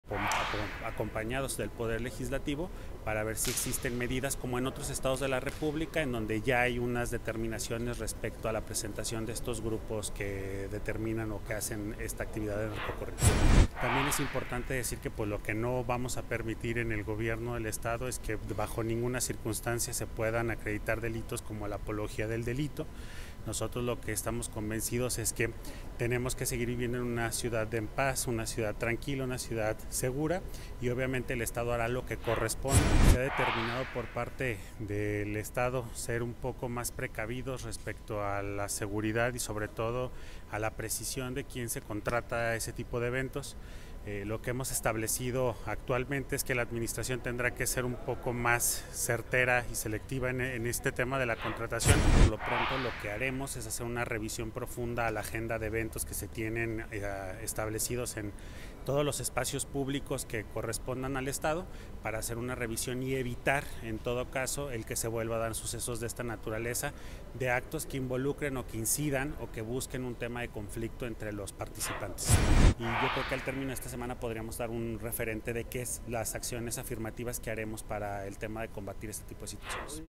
Gobierno del Estado busca prohibir presentación de grupos musicales especializados en narcocorridos
Aguascalientes, Ags. 17 de octubre 2022.- La actual administración estatal busca evitar a toda costa la presentación de grupos musicales especializados en narcocorridos, ante la apología del delito que realizan y las consecuencias negativas que acarrean, afirmó el Subsecretario General de Gobierno, Enrique García López.
En entrevista, refirió que trabaja de manera conjunta con el Congreso del Estado para establecer las medidas necesarias a efecto de que, como ya ocurre en otros estados de la República, se tomen determinaciones específicas para evitar las presentaciones de quienes, mediante su música, realicen apología del delito, lo cual es una violación a la ley.
Lo anterior, luego que la noche del sábado anterior un hombre muriera acribillado a balazos tras un pleito que inició precisamente durante un baile de este tipo, así como la serie de videos que han comenzado a circular en redes sociales en torno a hechos ocurridos dentro del Palenque de la Feria, donde se presentaron estos grupos.
Sobre el particular, el funcionario estatal dejó en claro que el Estado hará lo que sea necesario para que los hidrocálidos sigamos viviendo en una ciudad tranquila, para lo cual se revisará la agenda de eventos musicales y evitar así que se vuelvan a dar hechos de esta naturaleza.